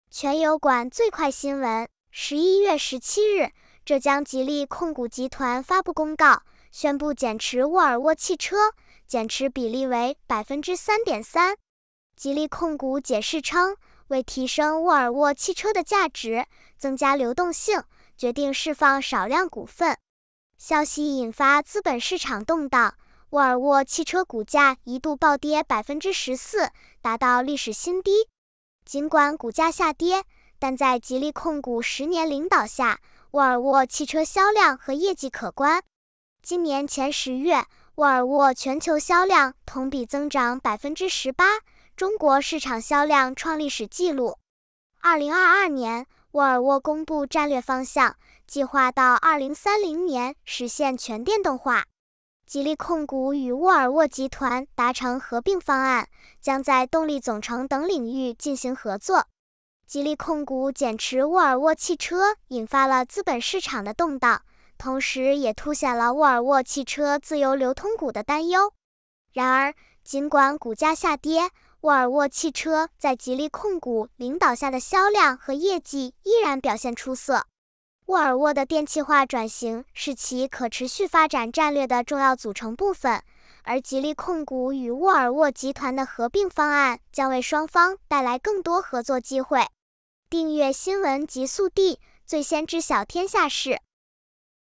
中国新闻11月19日10时：吉利出售沃尔沃部分股权，原因曝光
11月17日，浙江吉利控股集团发布公告，宣布减持沃尔沃汽车，减持比例为3.3%。吉利控股解释称，为提升沃尔沃汽车的价值，增加流动性，决定释放少量股份。消息引发资本市场动荡，沃尔沃汽车股价一度暴跌14%，达到历史新低。尽管股价下跌，但在吉利控股十年领导下，沃尔沃汽车销量和业绩可观。今年前10月，沃尔沃全球销量同比增长18%，中国市场销量创历史记录。2022年，沃尔沃公布战略方向，计划到2030年实现全电动化。吉利控股与沃尔沃集团达成合并方案，将在动力总成等领域进行合作。